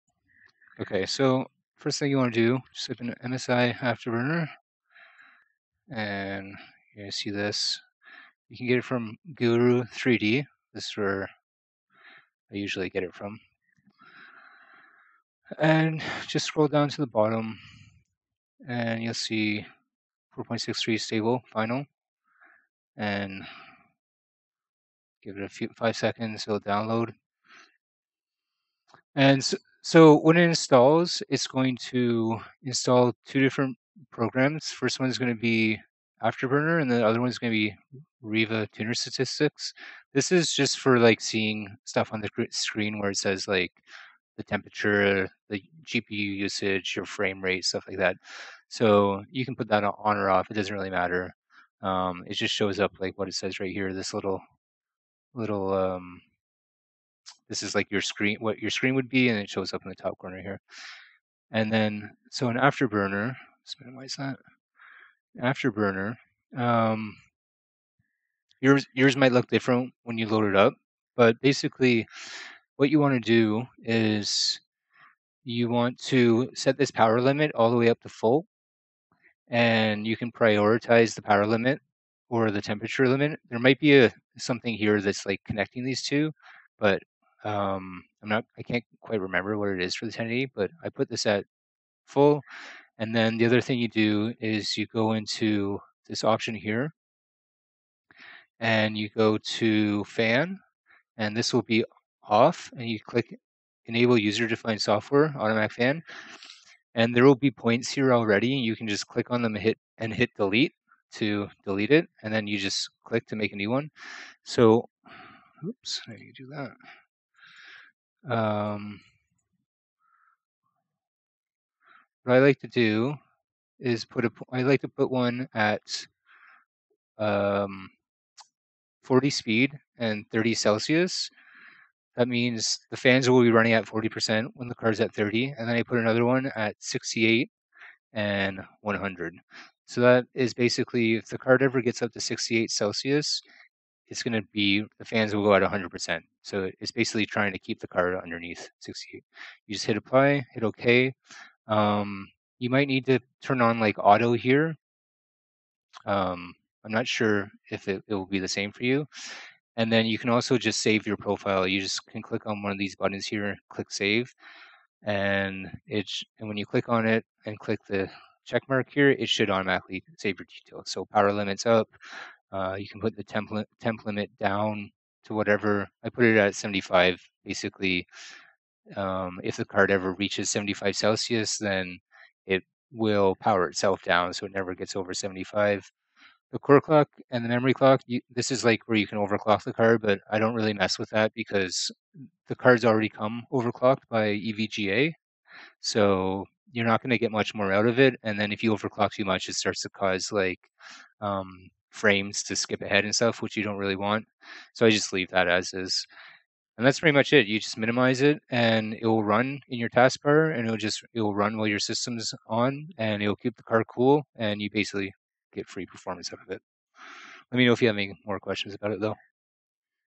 Basic quick setup for MSI Afterburner - keep your video card cool adjusting Power Limit & Temp Limit
nothing special - just the simple way to setup Afterburner without doing overclocks (look up other videos out there for that, there's tons of them) - just a straight forward basic setup.